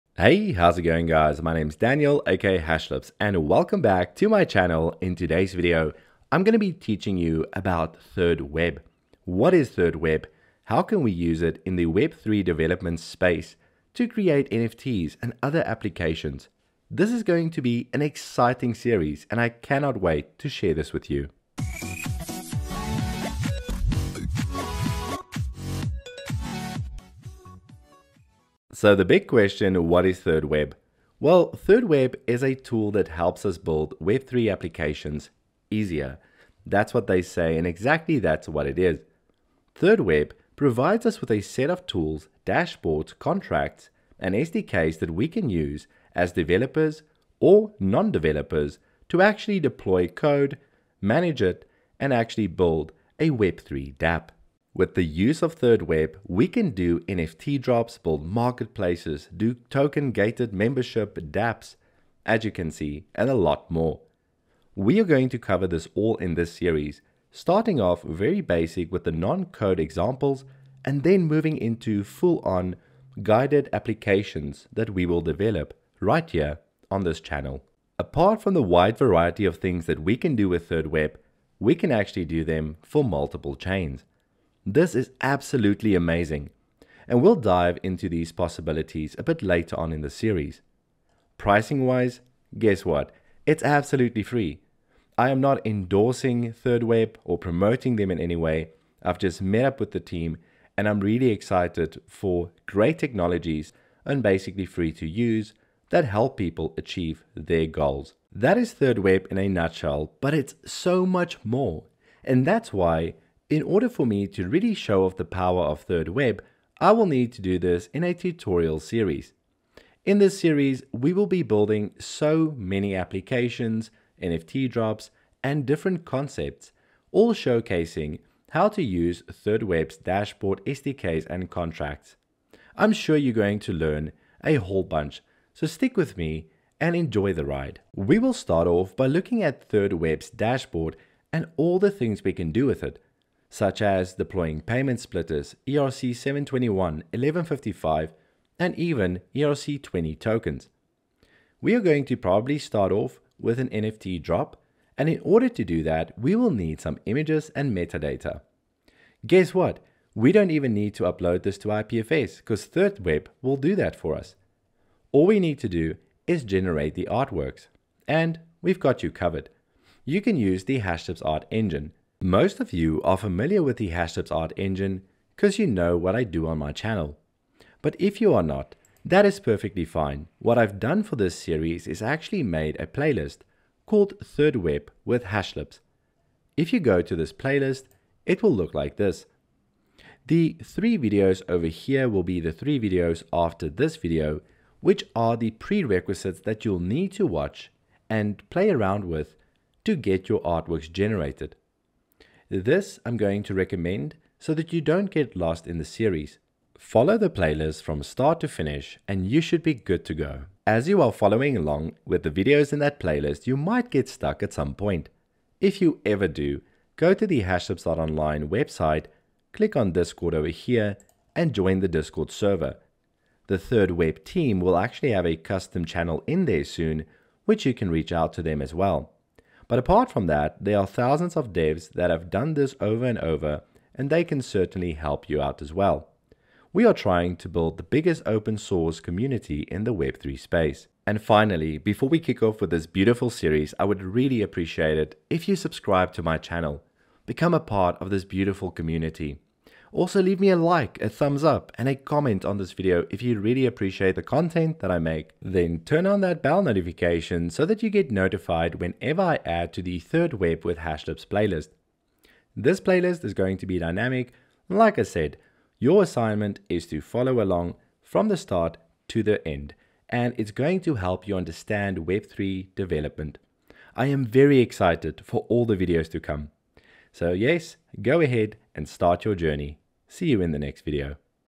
Build web3 apps, easily with thirdweb
This video forms part of a thirdweb series where I will explore how we can make use of thirdweb in the web3 space. We cover the introduction to the series. I had a lot of fun creating this video and I hope you have just as much fun following along.